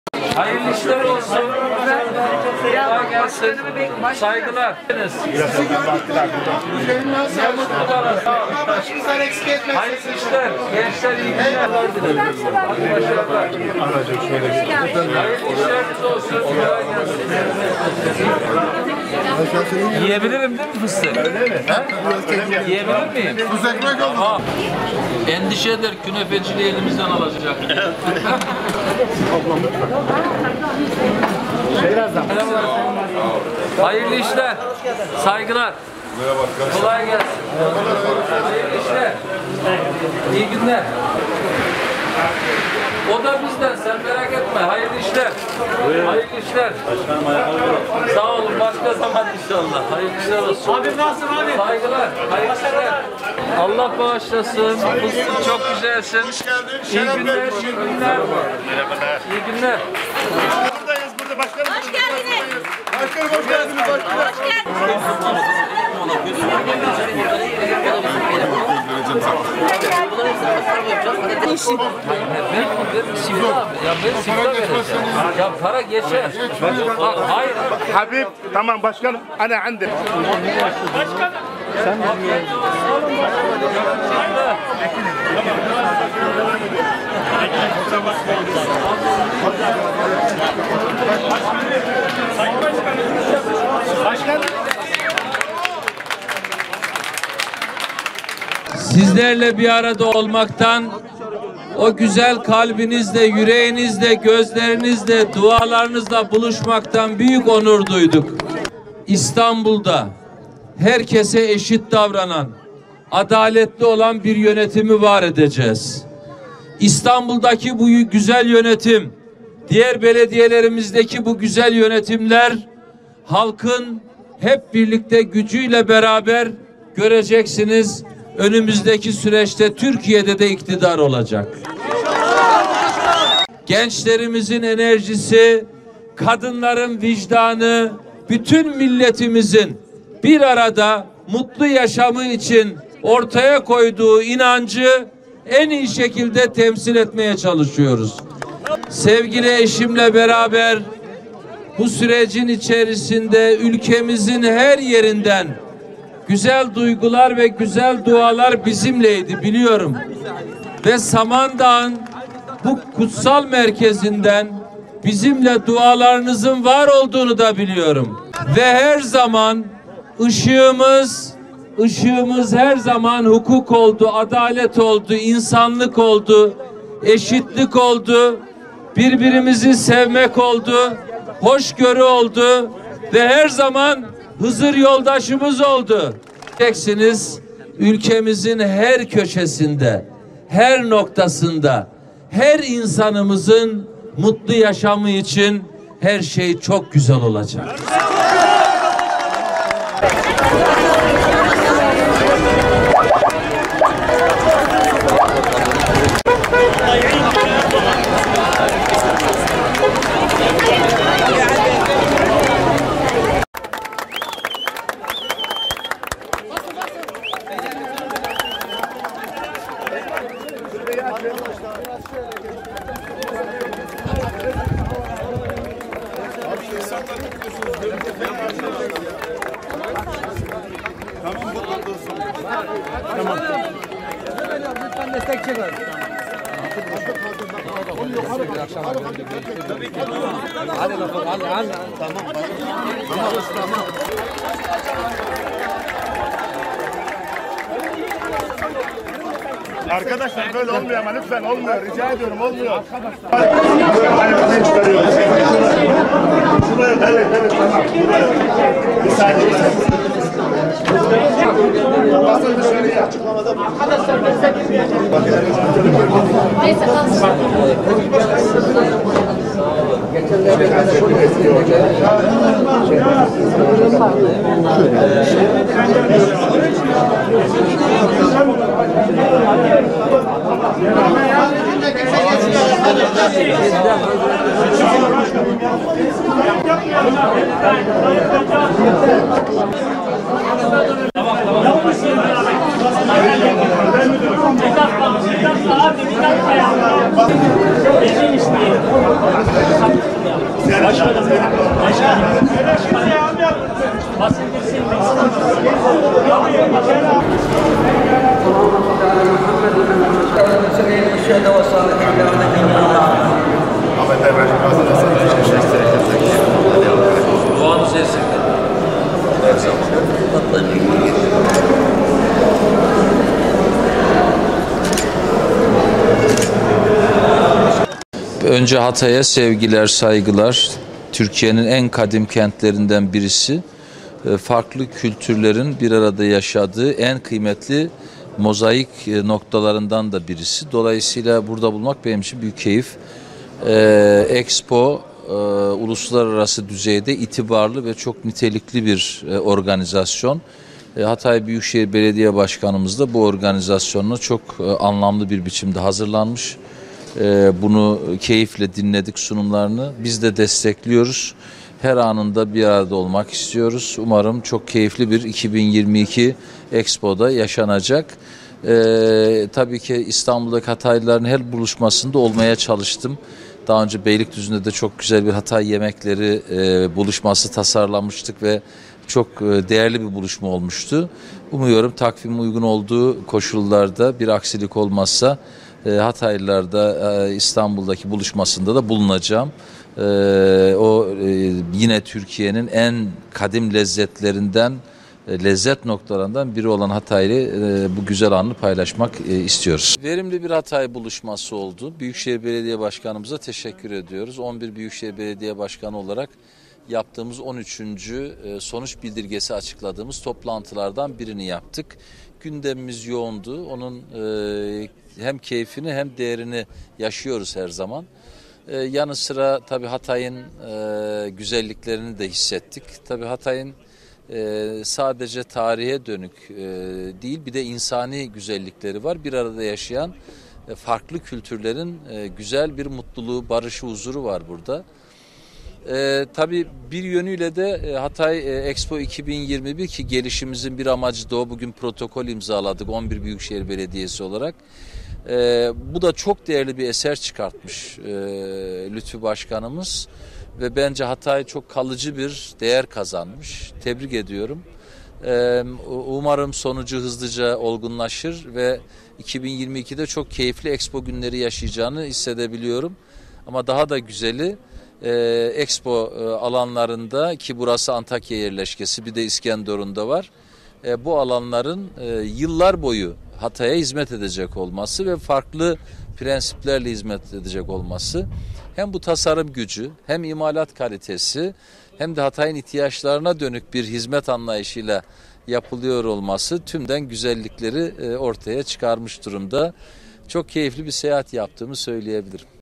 İmamoğlu'ndan büyük alkış alan konuşma! "İstanbul'daki güzel yönetim Türkiye'de de iktidar olacak!"
CHP’li Büyükşehir Belediye Başkanları Hatay buluşması için Hatay'da bulunan İstanbul Büyükşehir Belediye Başkanı Ekrem İmamoğlu, eşi Dilek İmamoğlu ile birlikte Samandağ ilçesine giderek Hz. Hızır türbesini ziyaret etti.

Samandağ halkının yoğun ilgisiyle karşılaşan İmamoğlu önce türbede dua etti. Türbe ziyaretinin ardından Samandağ kitap fuarının önüne kurulan platformun üzerine çıkarak Samandağ halkına hitap eden Ekrem İmamoğlu, “İstanbul'da herkese eşit davranan adaletli bir yönetim sistemini var edeceğiz” dedi.

Ekrem İmamoğlu, şunları söyledi:

“Sizlerle bir arada olmaktan, o güzel kalbinizle yüreğinizle, gözlerinizle, dualarınızla buluşmaktan büyük onur duyduk. İstanbul'da herkese eşit davranan adaletli olan bir yönetimi var edeceğiz. İstanbul'daki bu güzel yönetim, diğer belediyelerimizdeki bu güzel yönetimler, halkın hep birlikte gücü ile beraber göreceksiniz önümüzdeki süreçte Türkiye'de de iktidar olacak. Gençlerimizin enerjisi, kadınların vicdanı, bütün milletimizin bir arada, mutlu yaşamın için ortaya koyduğu inancı en iyi şekilde temsil etmeye çalışıyoruz. Sevgili eşimle beraber bu sürecin içerisinde ülkemizin her yerinden güzel duygular ve güzel dualar bizimleydi, biliyorum. Ve Samandağı’n bu kutsal merkezinden bizimle dualarınızın var olduğunu da biliyorum ve ışığımız her zaman hukuk oldu, adalet oldu, insanlık oldu, eşitlik oldu, birbirimizi sevmek oldu, hoşgörü oldu ve her zaman Hızır yoldaşımız oldu. Göreceksiniz ülkemizin her köşesinde her noktasında her insanımızın mutlu yaşamı için her şey çok güzel olacak.”